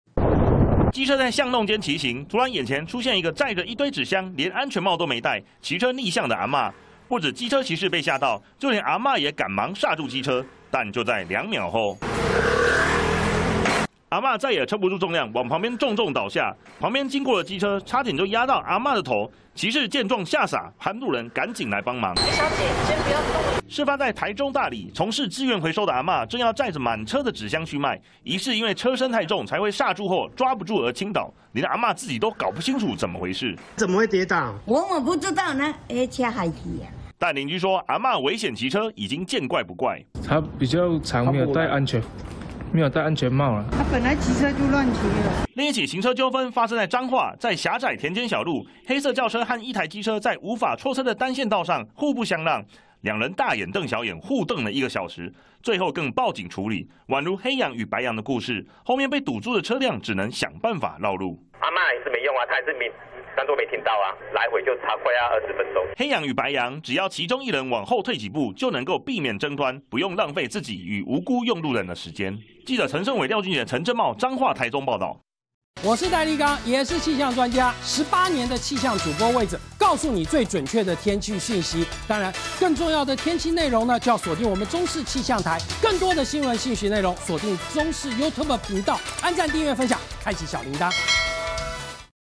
驚險! 嬤逆騎載回收箱 煞車倒地險被撞輾│中視新聞 20231216
驚險一瞬間!台中一名資源回收的阿嬤，騎車滿載一堆紙箱去賣，竟然逆向騎車，疑似車身太重，一煞車，就側身倒地，這時，剛好一台機車經過，差點被撞上輾過!另外，黑羊白羊真實版上演，在彰化永靖一條小路，只容得下一台車走，兩駕駛互不相讓，造成交通堵塞1小時!